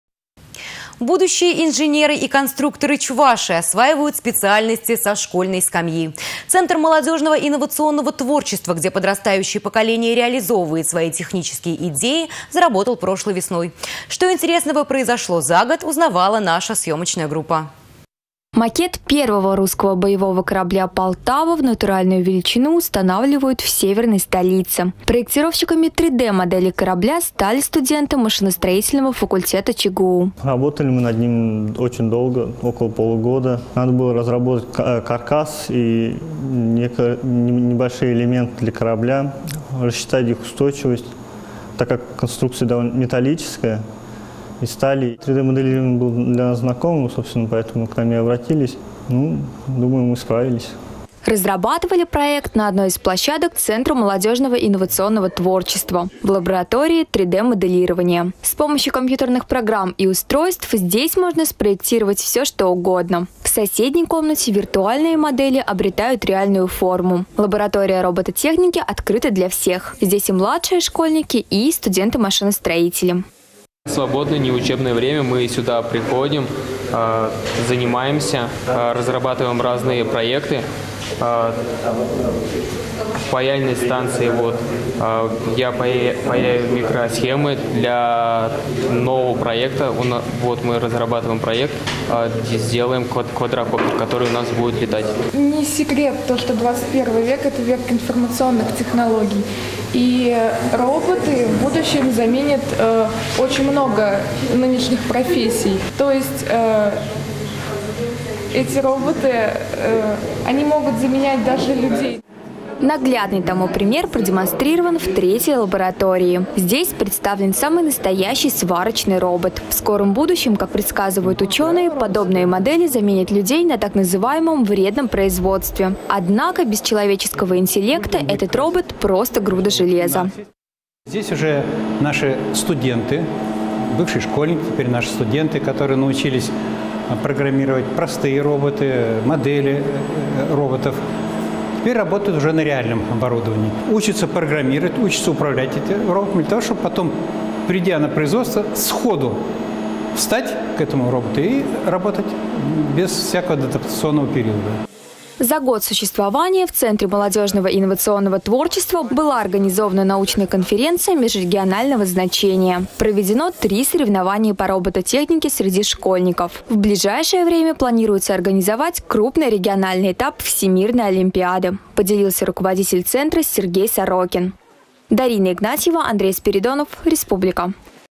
ЦМТИ Чувашской Республики один год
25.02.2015 Будущие инженеры и конструкторы Чувашии осваивают специальности со школьной скамьи. Центр молодежного инновационного творчества, где подрастающее поколение реализовывает свои технические идеи, заработал прошлой весной.